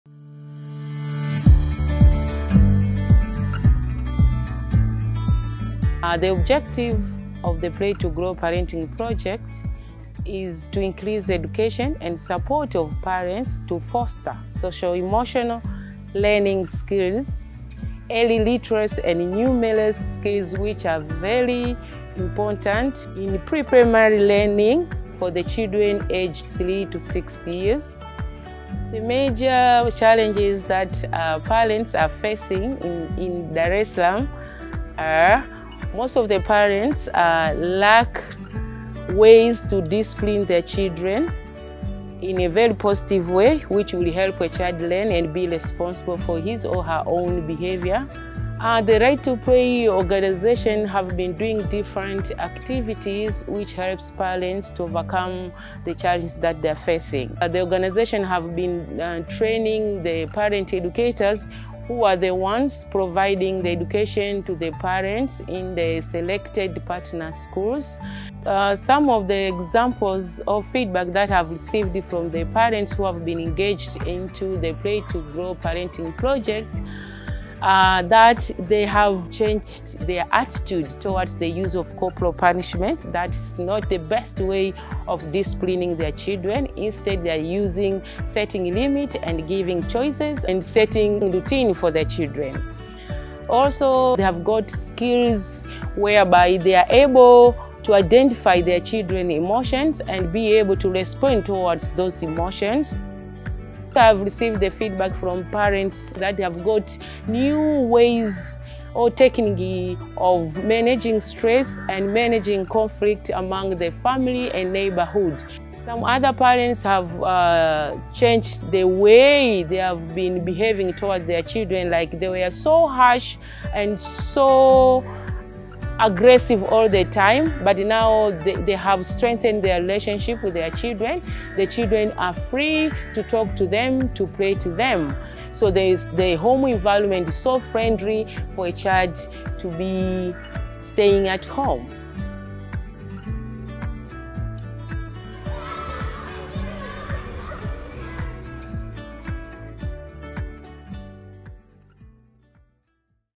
Supporting Parents to Foster Social Emotional Learning Skills in Tanzania
Corporal punishment remains common among both parents and teachers in Tanzania as a way to discipline children.

Field Facilitator for our Play to Grow project, Monica Millinga, explains how we're combatting that harmful practice and providing positive alternatives of discipline for parents, along with tools to be able to better respond to their children's emotions.

This project is taking place thanks to the generous support of  @thelegofoundation